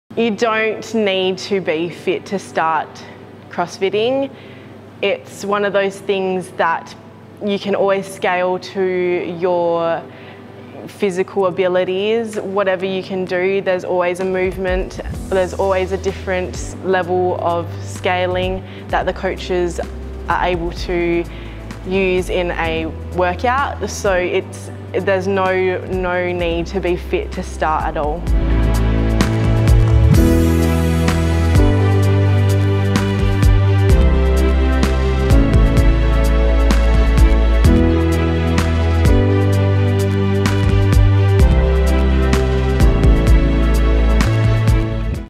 Des, You Dont have to be fit
Desiree briefly explains that you don't have to be fit to start CrossFIt